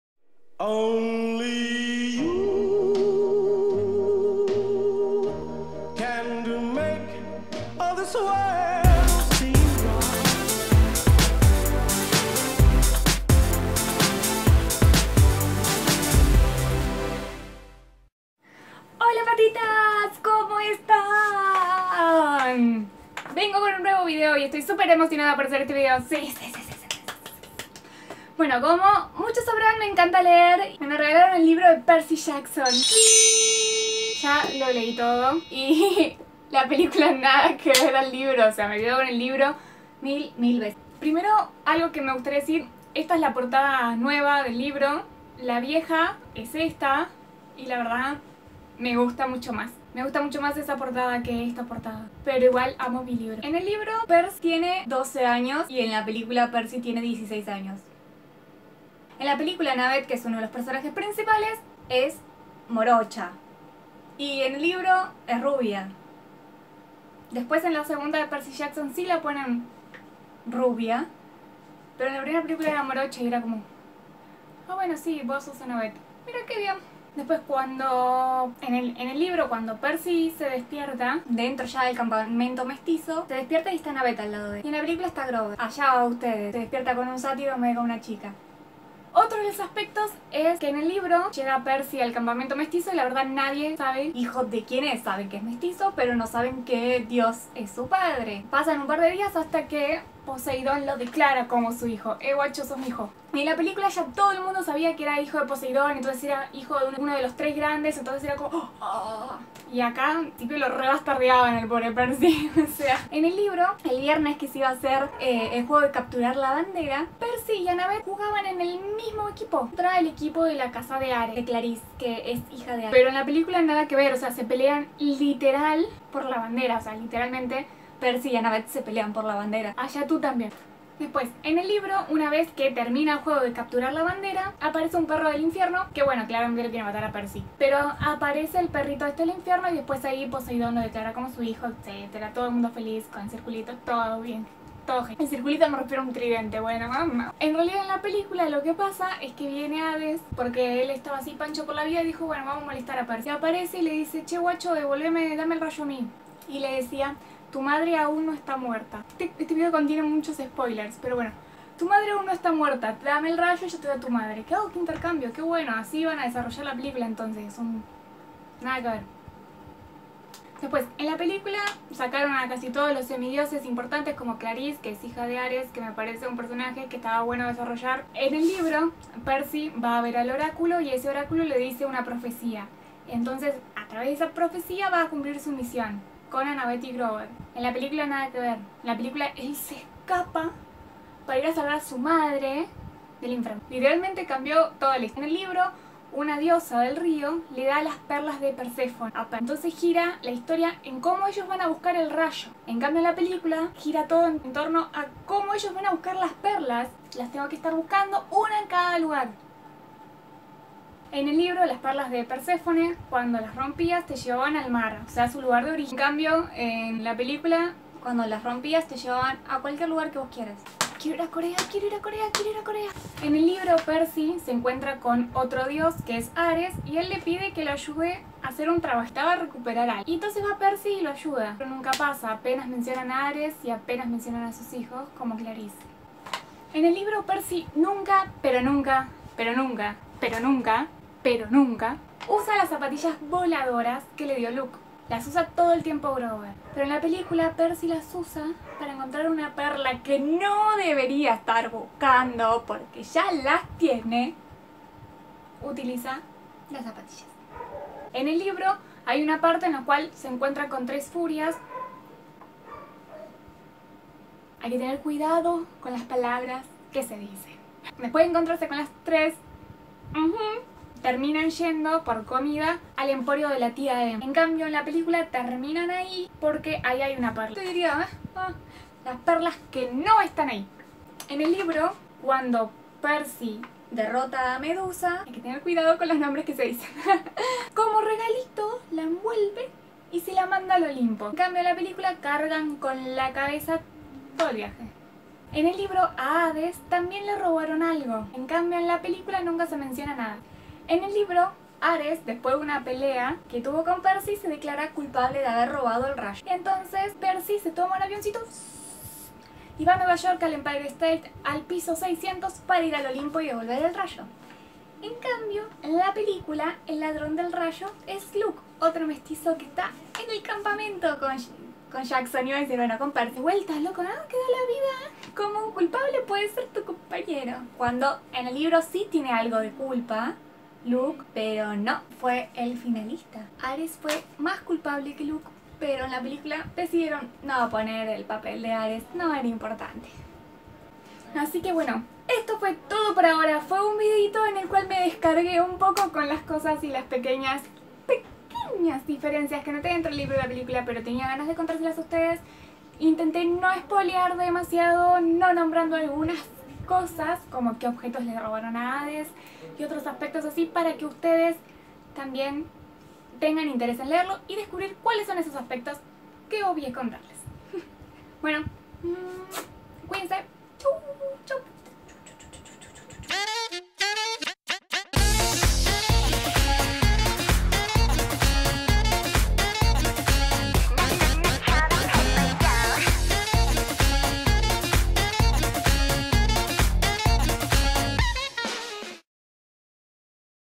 [Review] Percy Jackson: El Guión perdido - Book vs. Film|Paw Depp
Hola! hace mucho no hacia una review. Decidí ponerle este nombre al vídeo porque creo que el director de la película de verdad perdió el guion e improviso sobre la marcha... jajaja

CUIDADO CONTIENE SPOILER si no viste la película o leíste el libro.